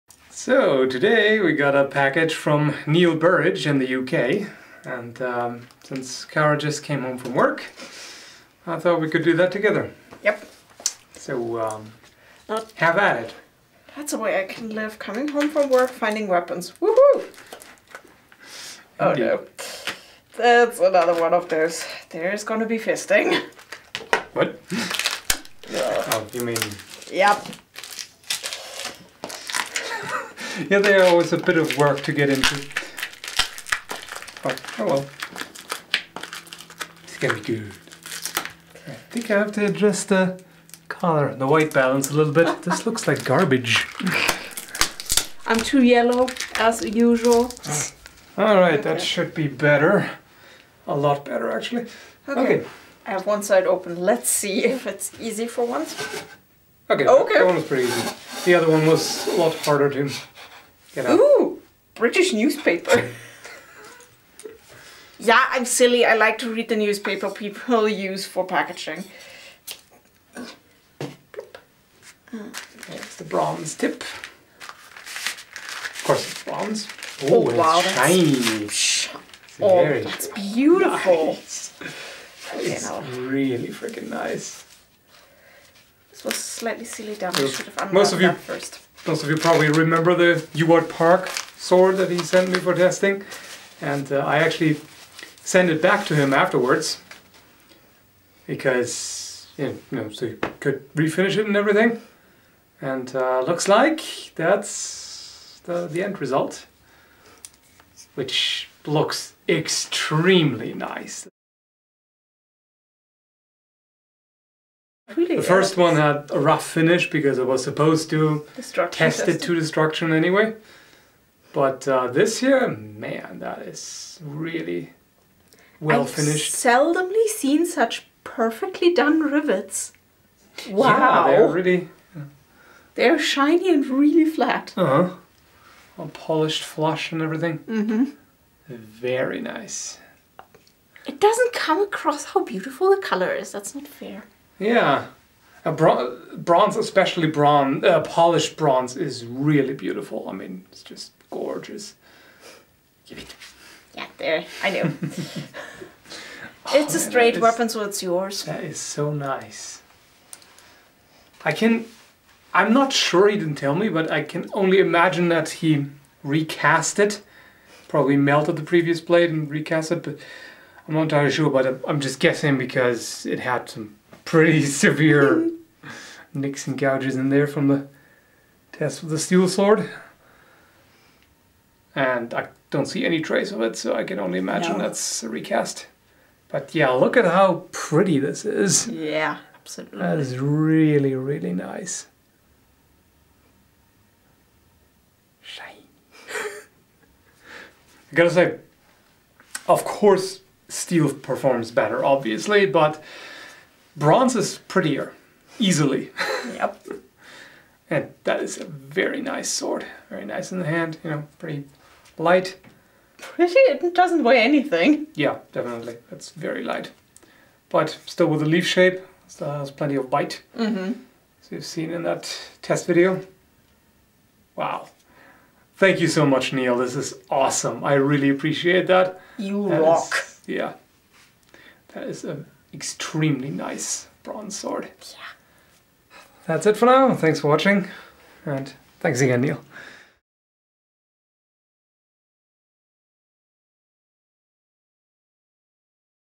Unboxing the re-finished Ewart Park bronze sword
After I did the destructive testing of the Ewart Park sword I sent it to Neil Burridge for repair or recasting. What he sent back is just absolutely beautiful. Polished bronze is amazingly beautiful, and this is a very well made historical replica, true to the originals.